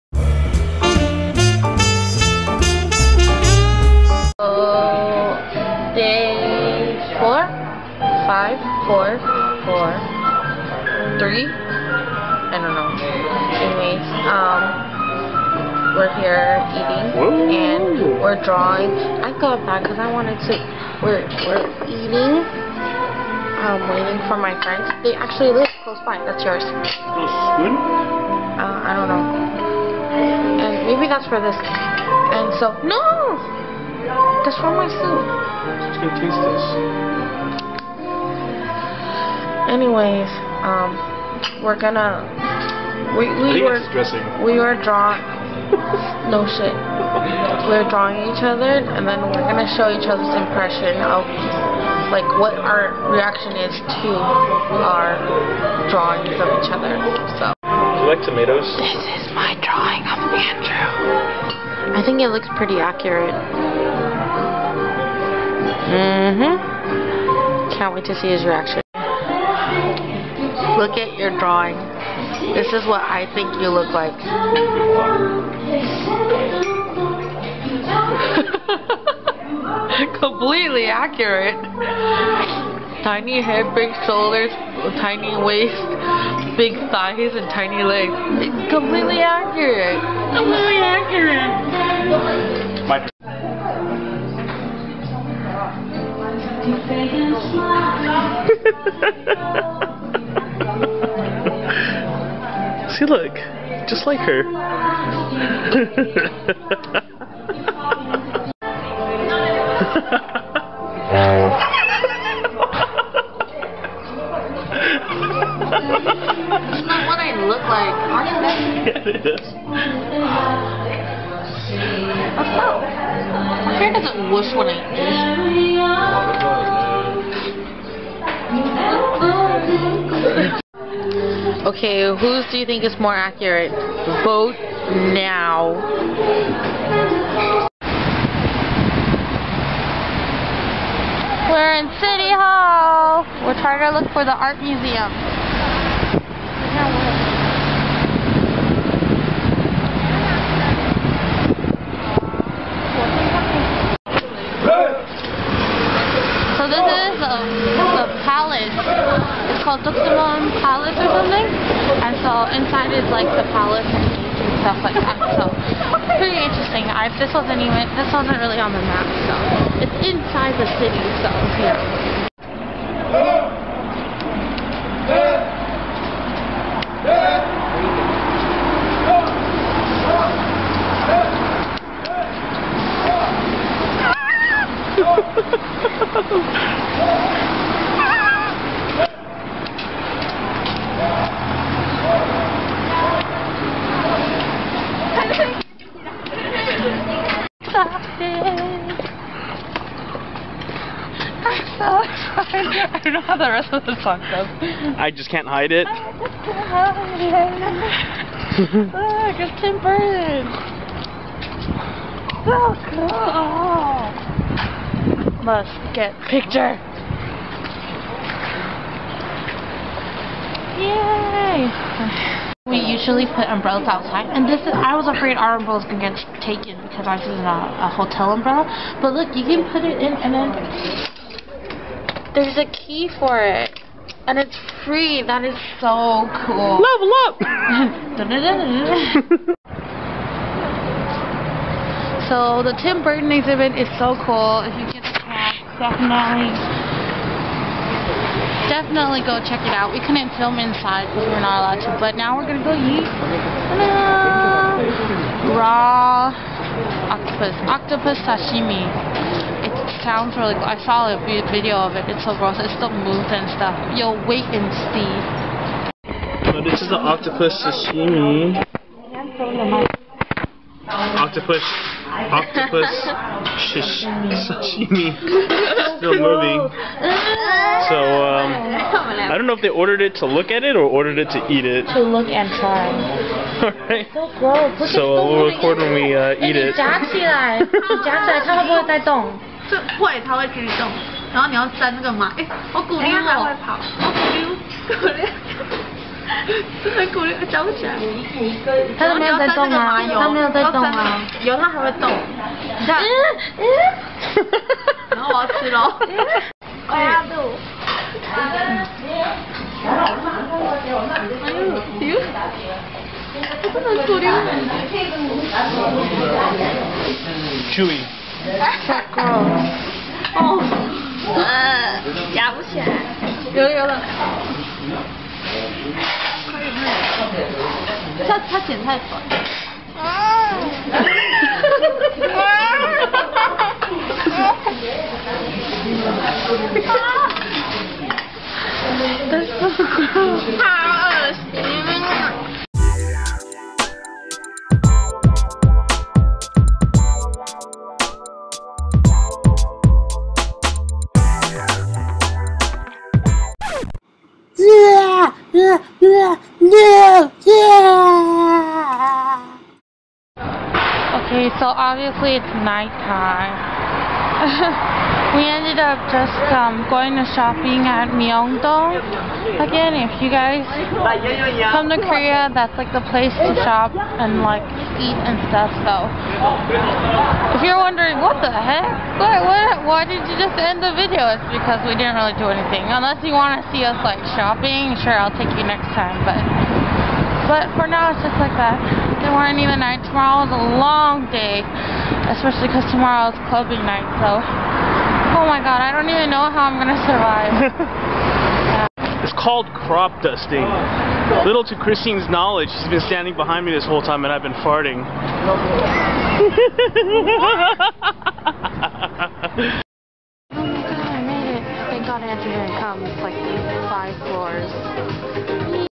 itFOBtimes meets Tim Burton! (Korea vLog)
Its day six of rainy korea until we meet tim burton with octopus limbs! Keep watching to see what happens.